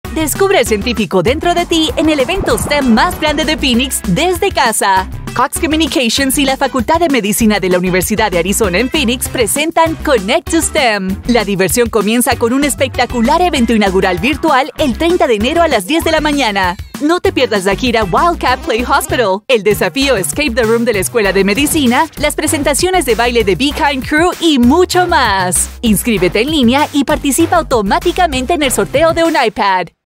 ¡Connect2STEM Regresa el 30 de Enero!
Cox y la Facultad de Medicina en Phoenix de University of Arizona serán anfitriones de CONNECT2STEM en el sábado 30 de enero.

Regístrate en phoenixmed.arizona.edu/c2s para una oportunidad de ganar un iPad.